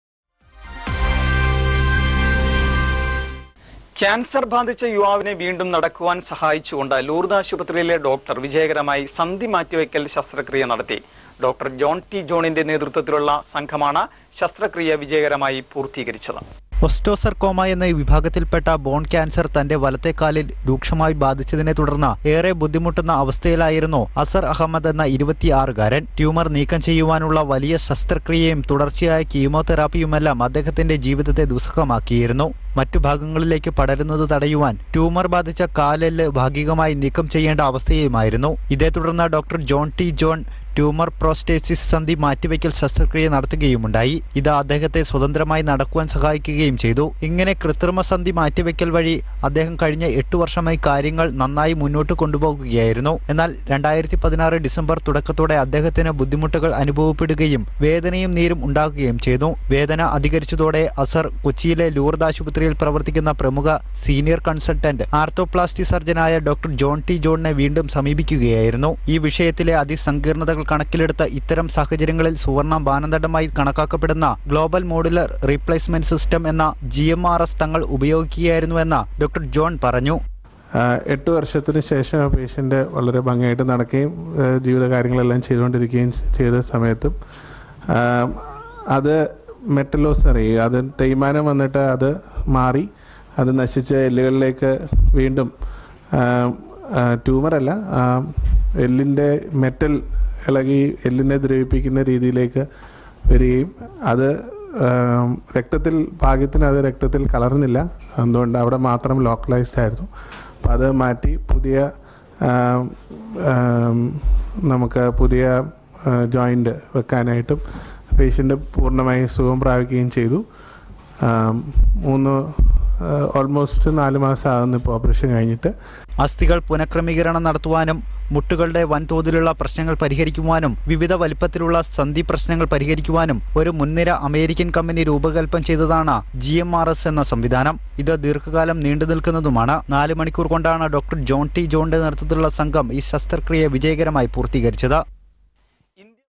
Press Release on Rare Bone Surgery by Dr. John Thayyil John | Lourdes Hospital Kochi
Rare Bone Surgery Performed by Dr. John Thayyil John (Press Meet)